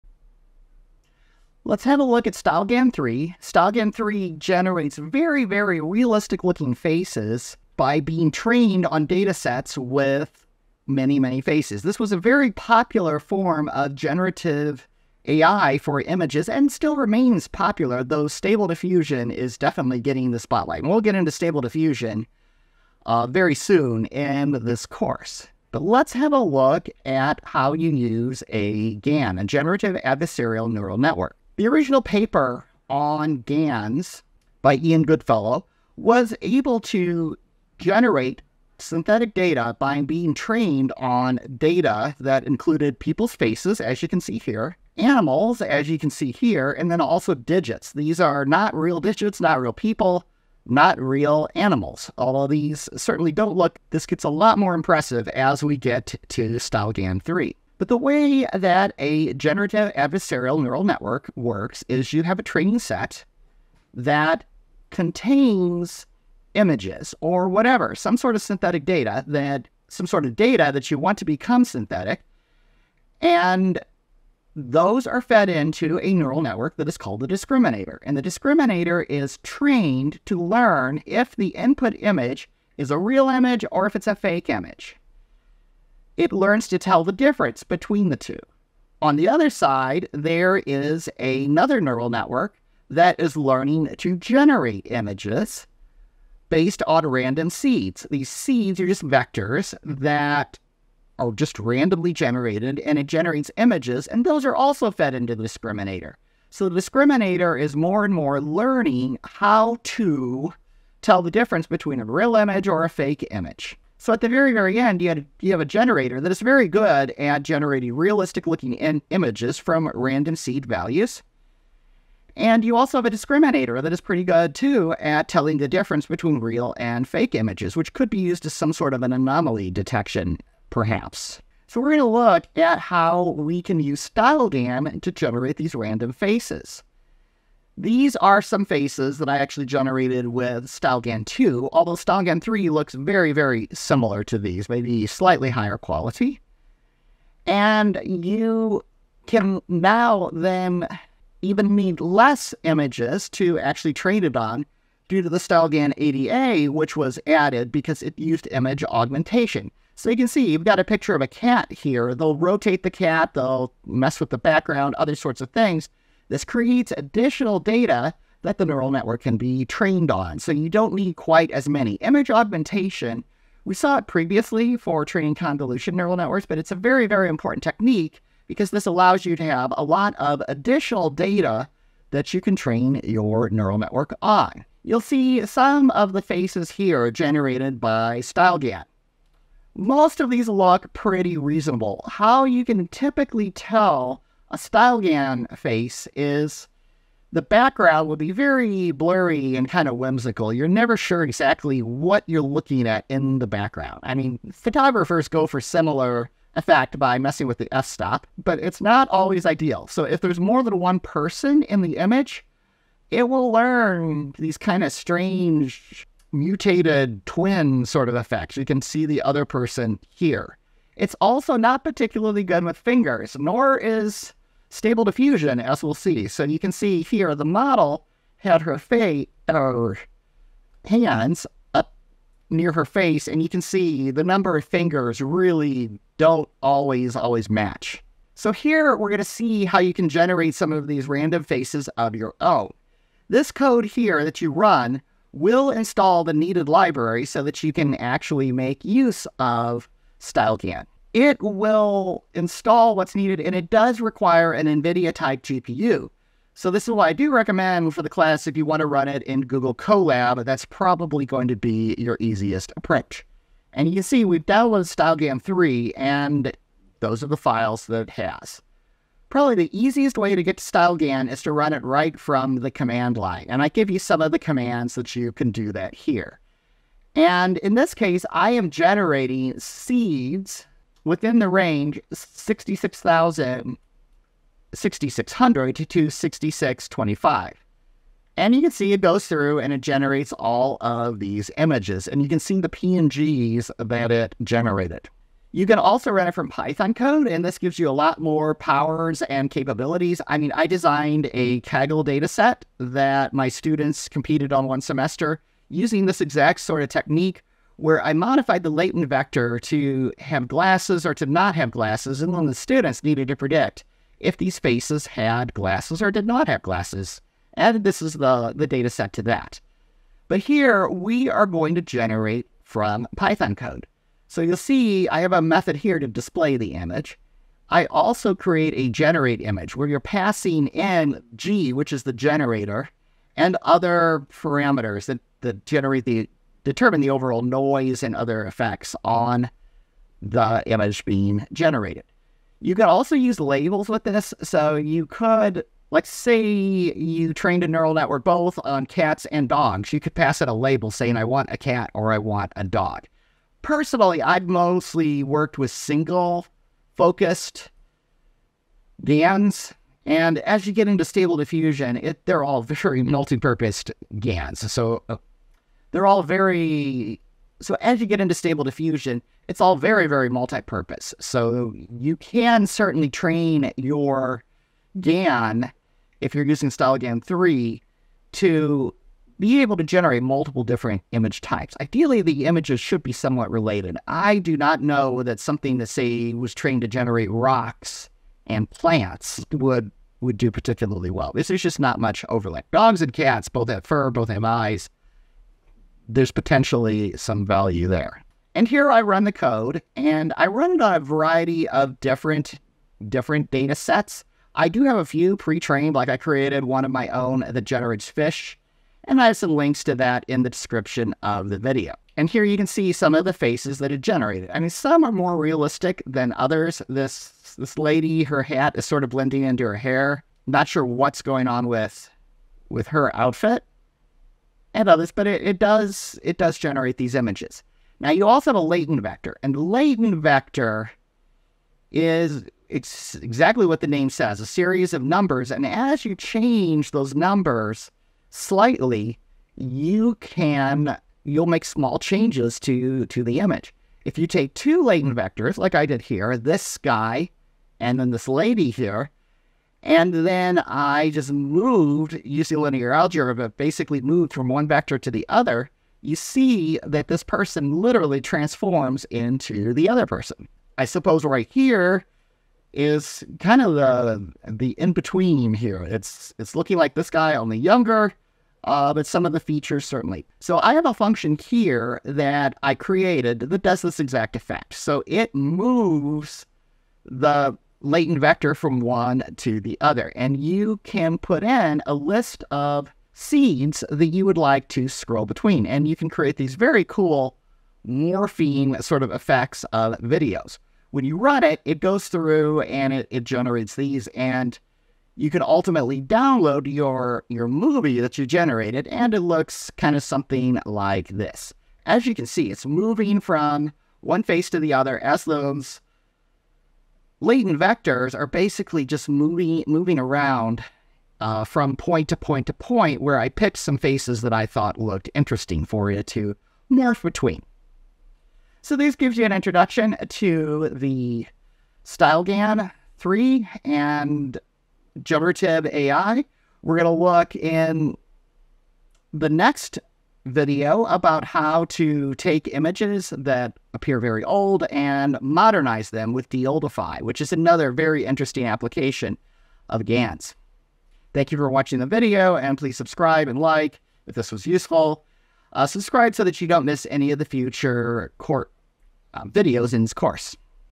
Generating Faces with StyleGAN3 (7.2)
This video introduces innovative realm of AI with our exploration of NVIDIA's groundbreaking StyleGAN3. Building on its predecessors, StyleGAN3 promises unparalleled precision, efficiency, and adaptability in the domain of generative adversarial networks. Witness firsthand the leaps made in AI-powered image generation, as we unpack the architecture enhancements, demonstrate its awe-inspiring capabilities, and discuss its transformative potential for various industries. Stay ahead of the curve with this glimpse into the future of generative modeling.

COURSE MATERIAL
📖 Textbook - Coming soon